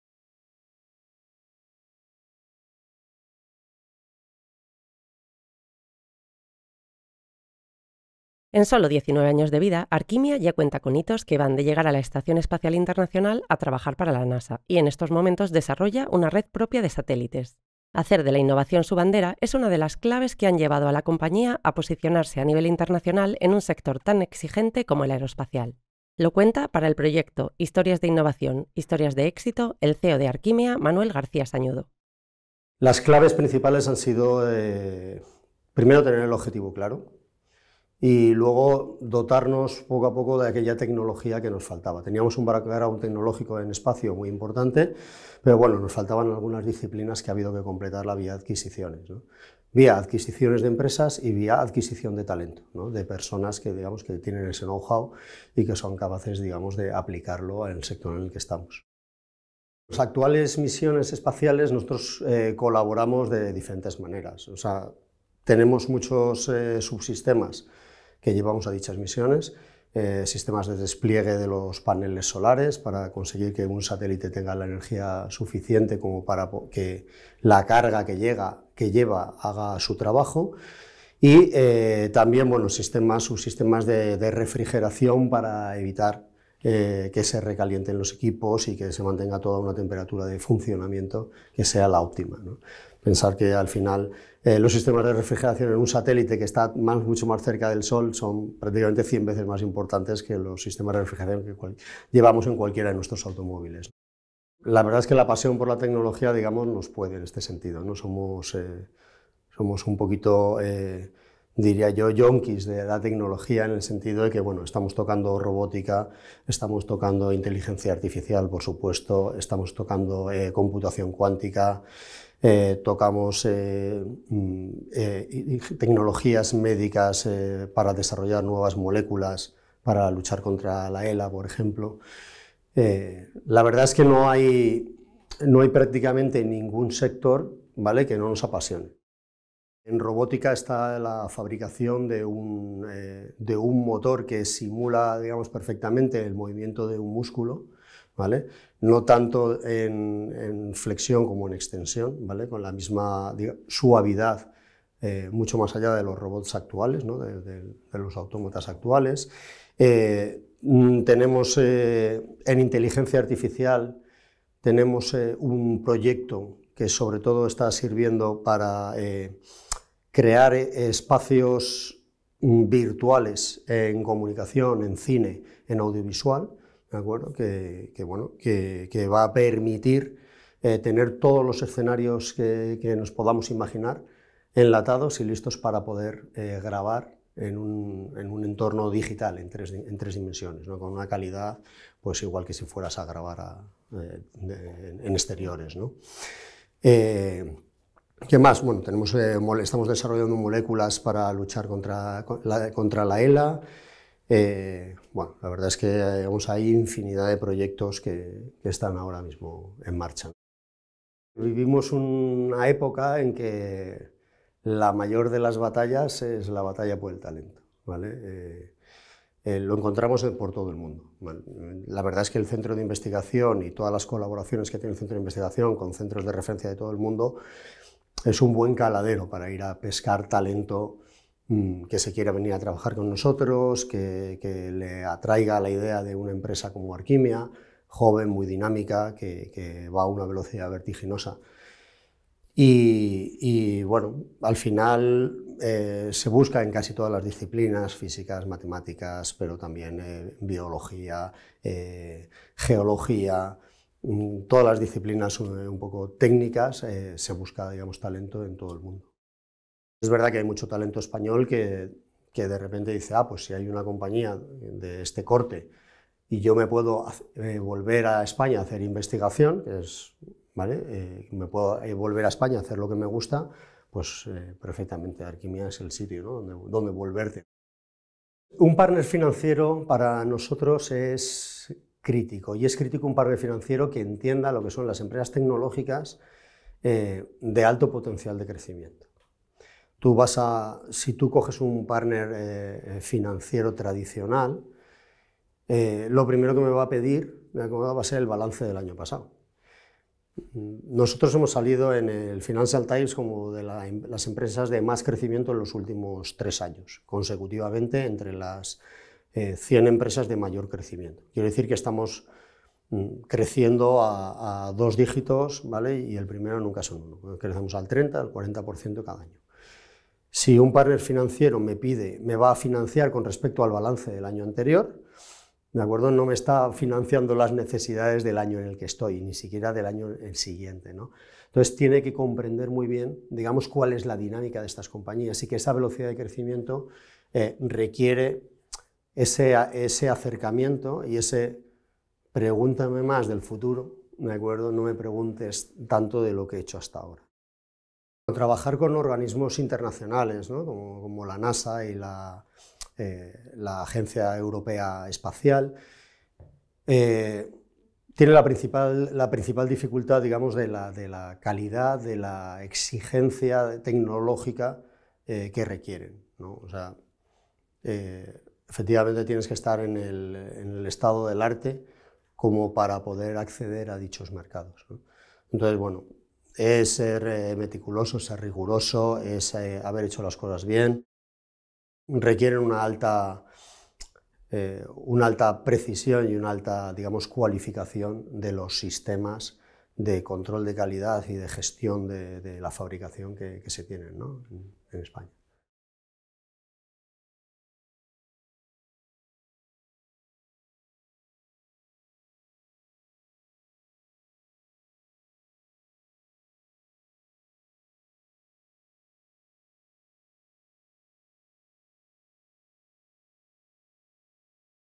Arquimea, la empresa que lleva alta tecnología española al espacio
Su tecnología ha viajado a la Estación Espacial Internacional e, incluso, a Marte, a bordo del último vehículo que la NASA envió al planeta rojo.
Haciendo de la innovación su bandera, Arquimea ha conseguido convertirse en una referencia a nivel internacional en un sector tan exigente como el aeroespacial.